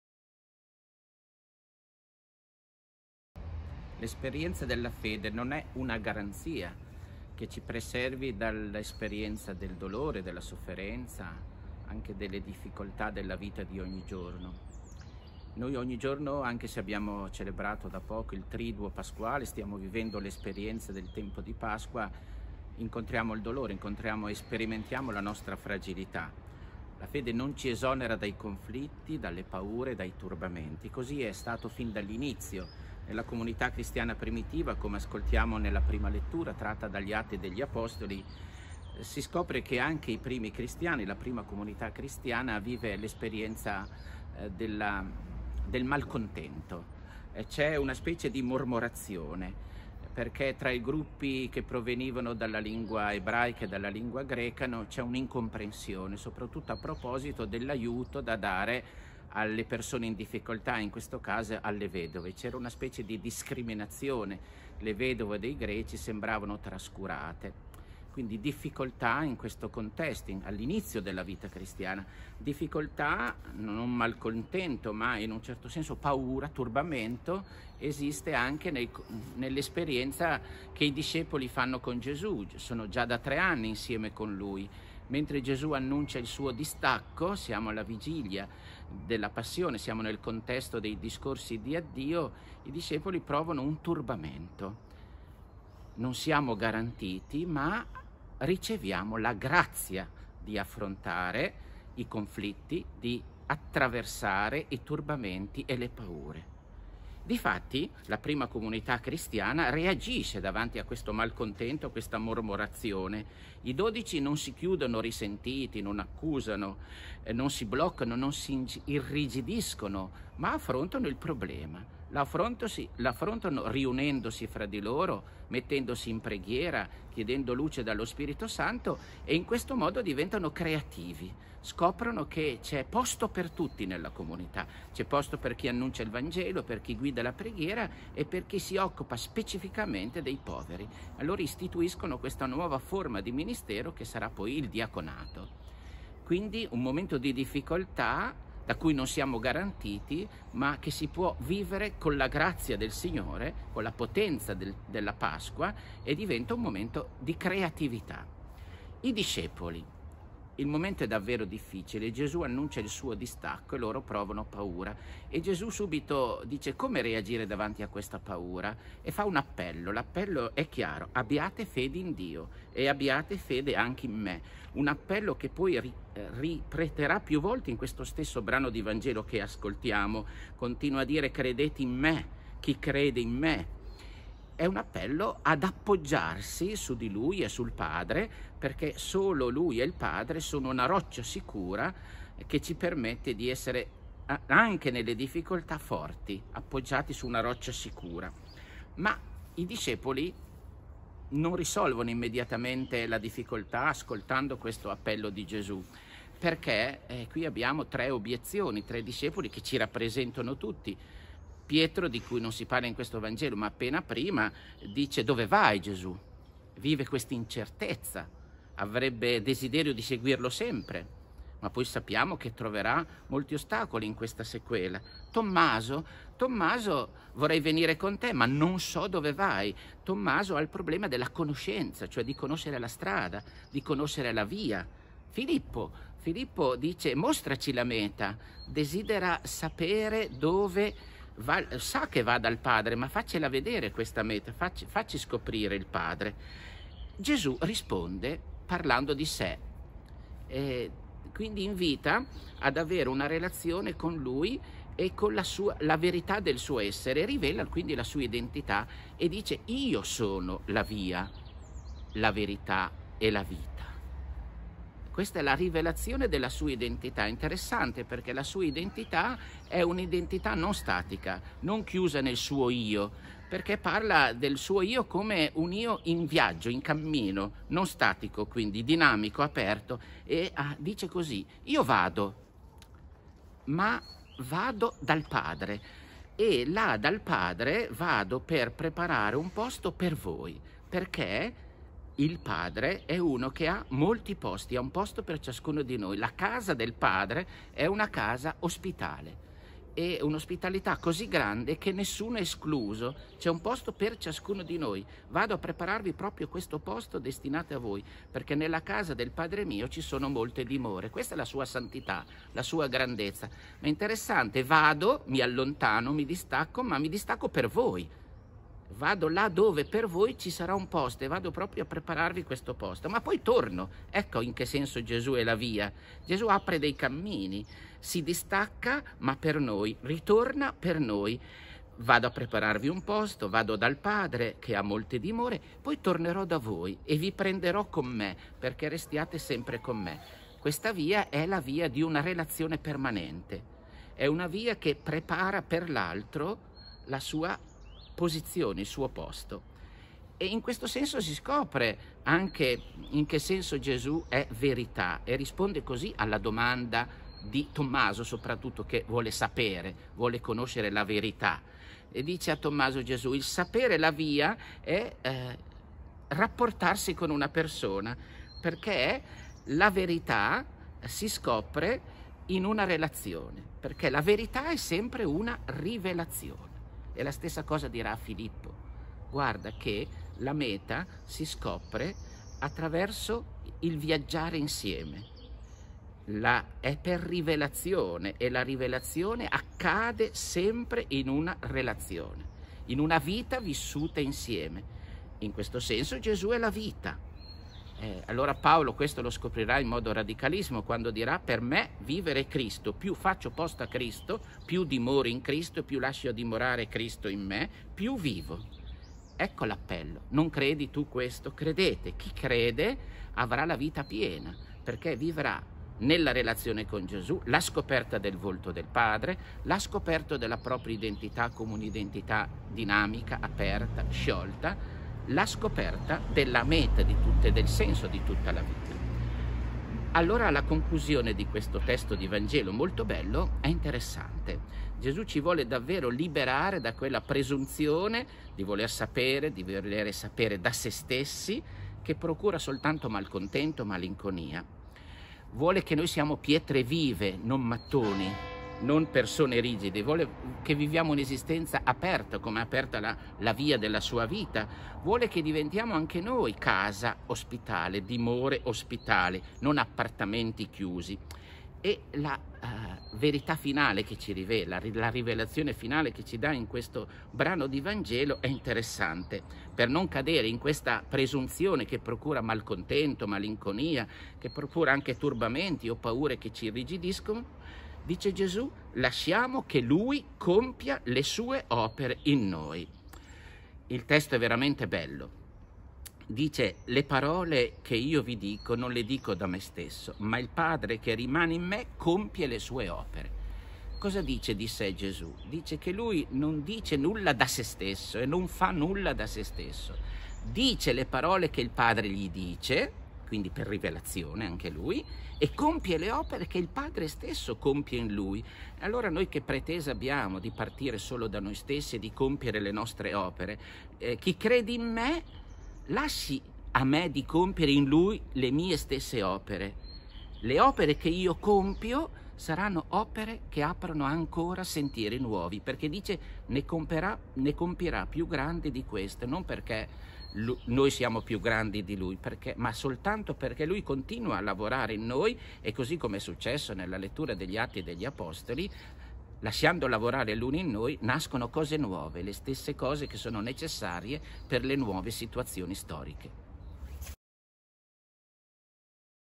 Vangelo di domenica 10 maggio 2020 - V di Pasqua
Mons. Giuseppe Busani, parroco della parrocchia di Castel San Giovanni, commenta il vangelo festivo